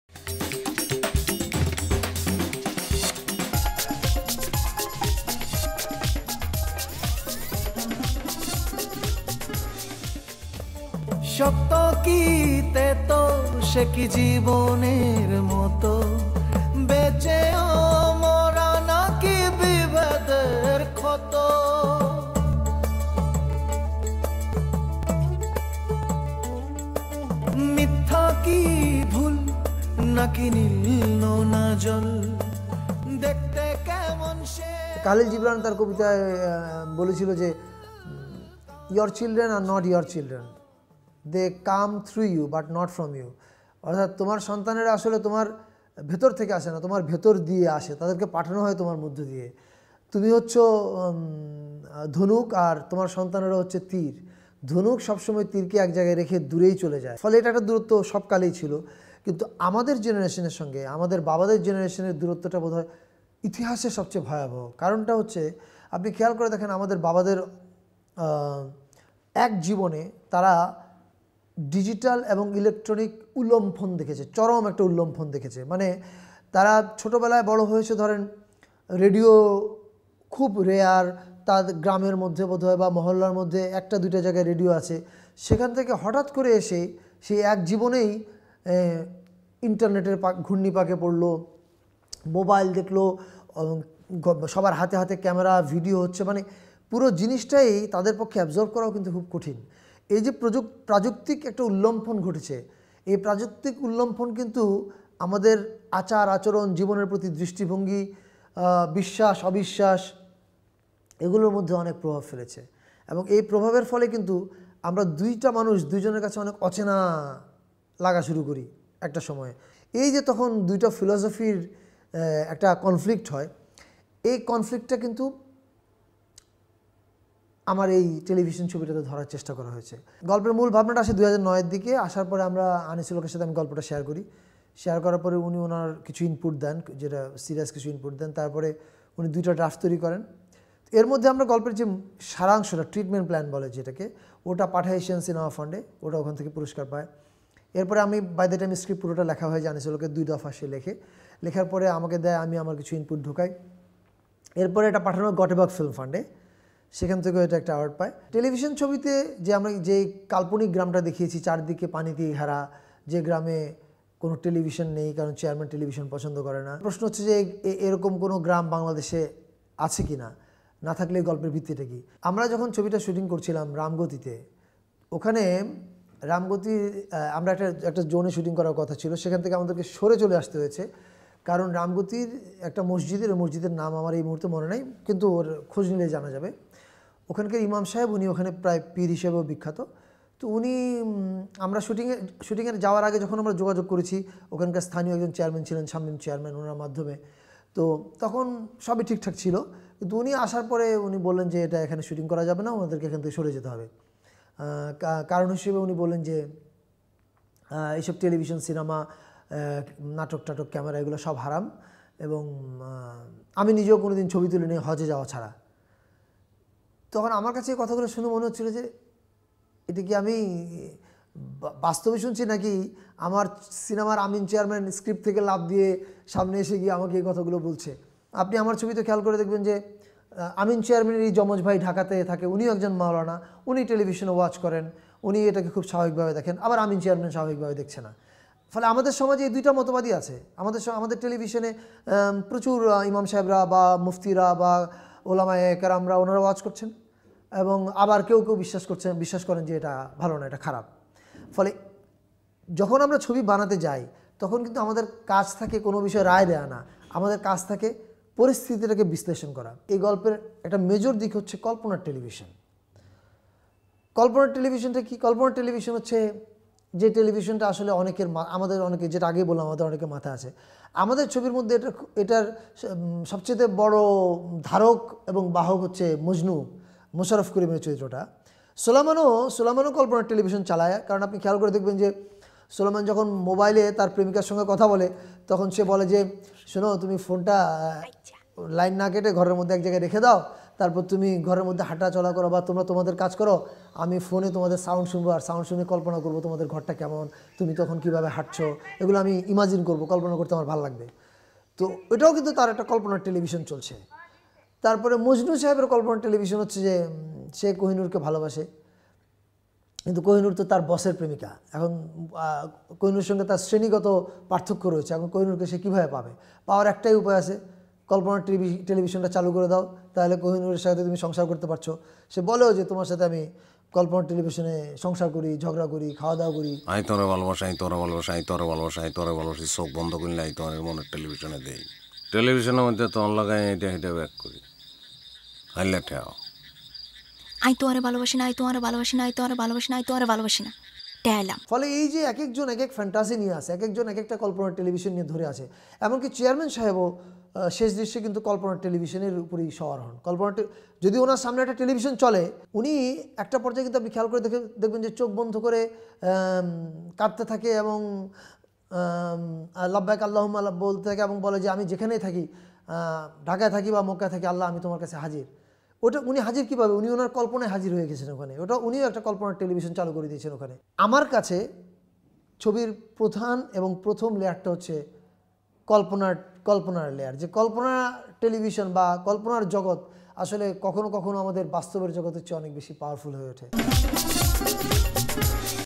Making of TELEVISION Part 1
Interview of Mostofa Sarwar Farooki about the Film 'TELEVISION'